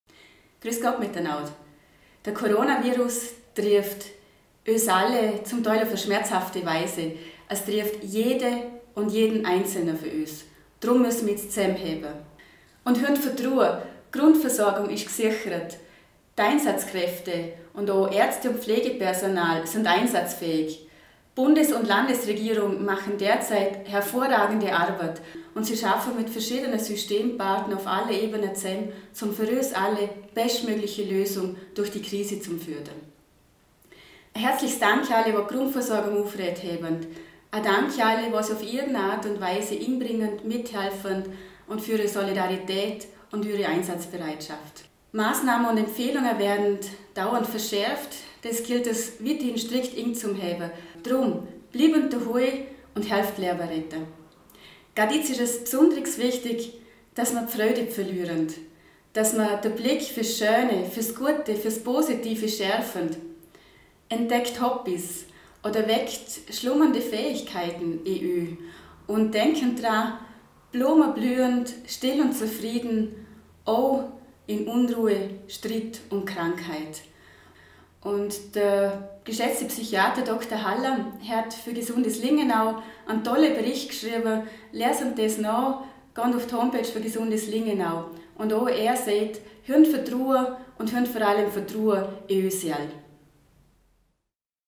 Covid-19: Lingenauer Bürgermeisterin Annette Sohler mit Videobotschaft
VOL.AT gibt Vorarlberger Bürgermeistern die Chance, sich per Videobotschaft an die Bürger zu wenden. So denkt Annette Sohler, Bürgermeisterin von Lingenau über die derzeitige Situation.